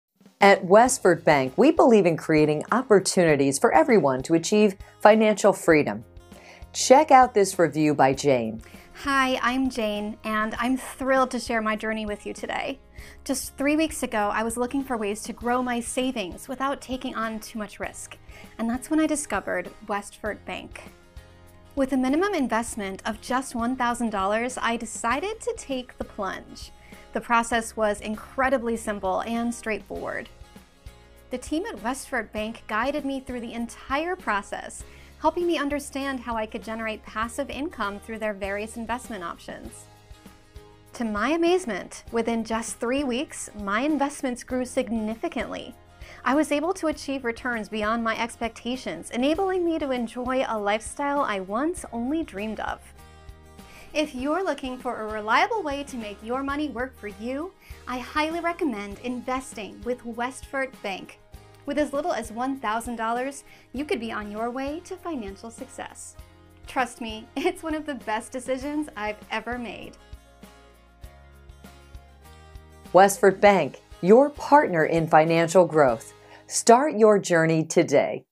Investor's Journey: How I started Using Westfort Bank for Success
Join us as successful investor Jane shares her inspiring journey of how she began using Westfort Bank to elevate her investment strategies and achieve financial success. Discover the pivotal moments that led her to choose Westfort Bank, the unique services and features that have supported her growth, and valuable insights for anyone looking to enhance their own financial journey. Whether you're an aspiring investor or looking for a reliable banking partner, this testimonial will provide you with the motivation and knowledge to make informed decisions. Don't miss out on this compelling story of growth and success!